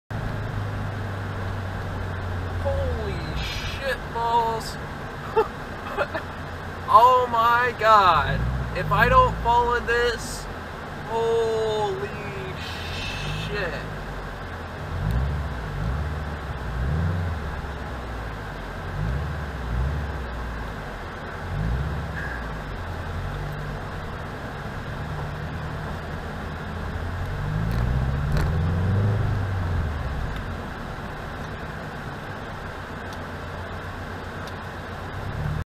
Braving a Huge Pot Hole
Pot hole grew after large amount of rain at Lost Creek. Car is lowered so could be deadly for undercarriage.